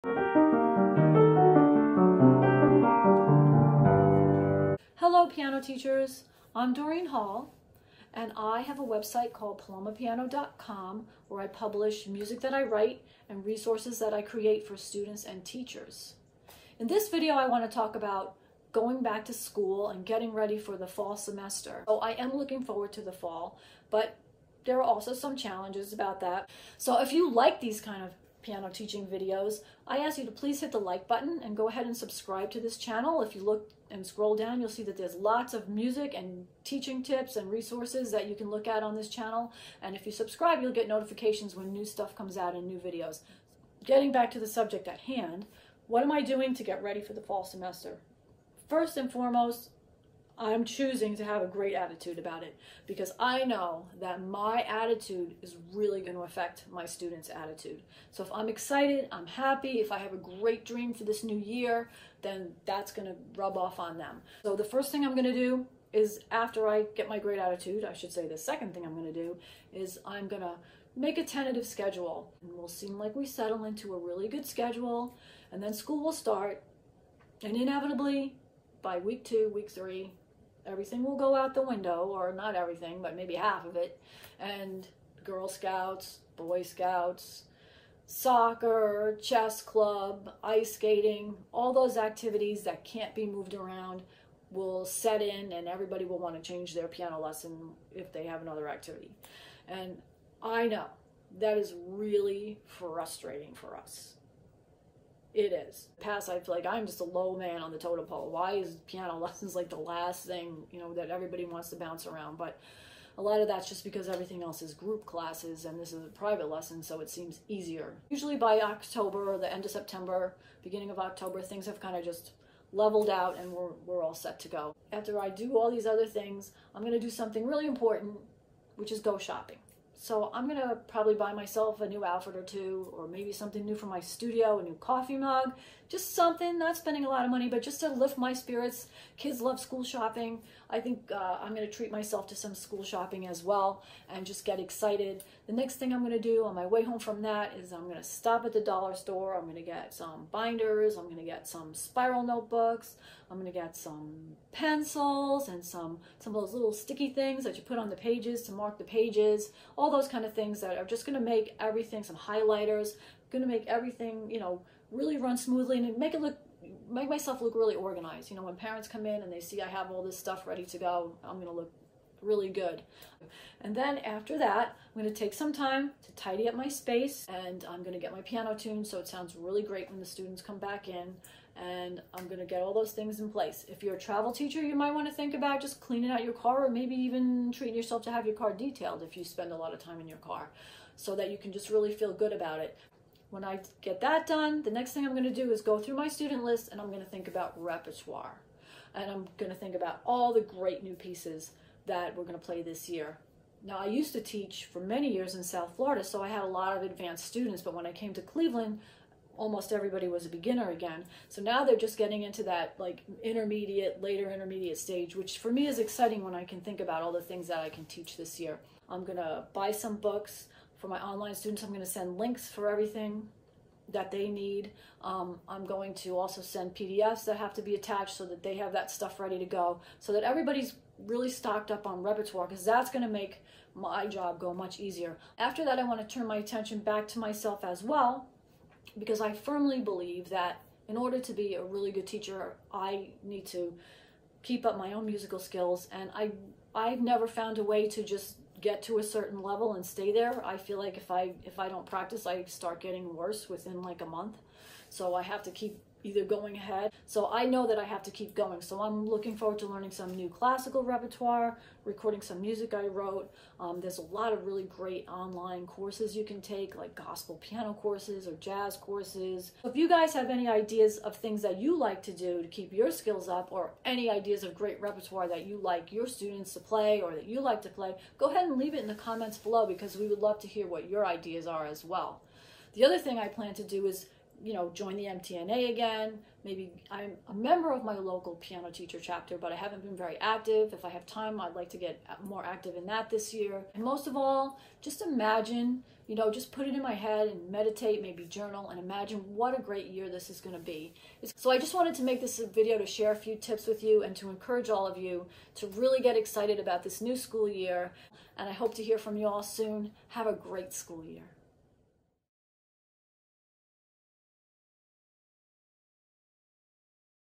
Let's get back to school. Tips for making back to school easier for piano teachers.
Make getting back to school a great time of year.